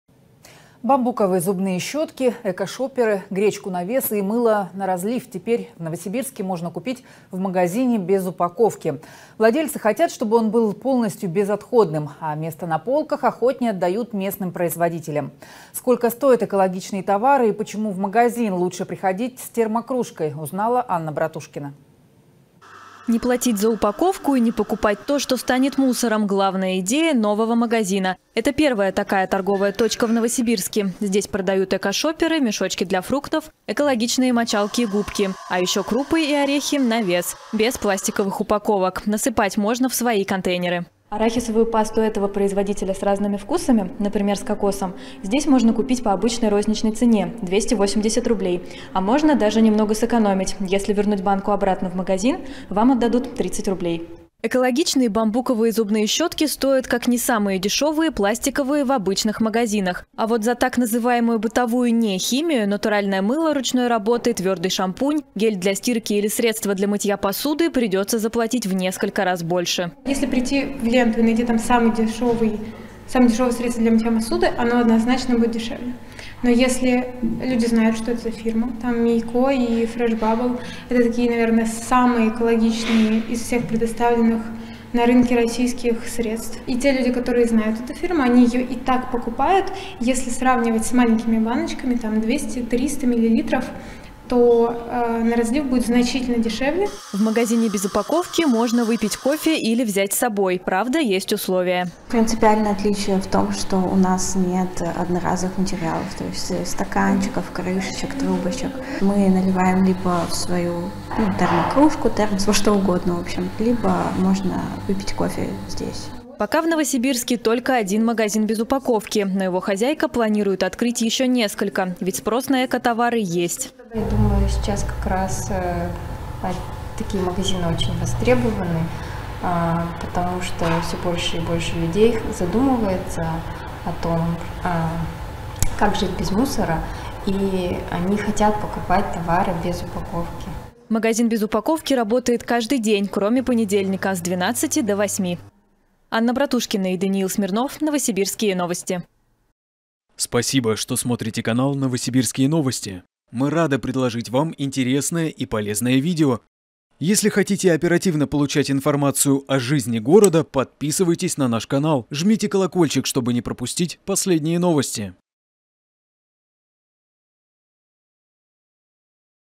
Необычный экомагазин с товарами без упаковок открыли в Новосибирске
Бамбуковые зубные щётки, экошоперы, гречку на вес и мыло на розлив теперь в Новосибирске можно купить в магазине без упаковки. Владельцы хотят, чтобы он был полностью безотходным, а место на полках охотнее отдают местным производителям. Сколько стоят экологичные товары и почему в магазин нужно приходить с термокружкой — узнали «Новосибирские новости».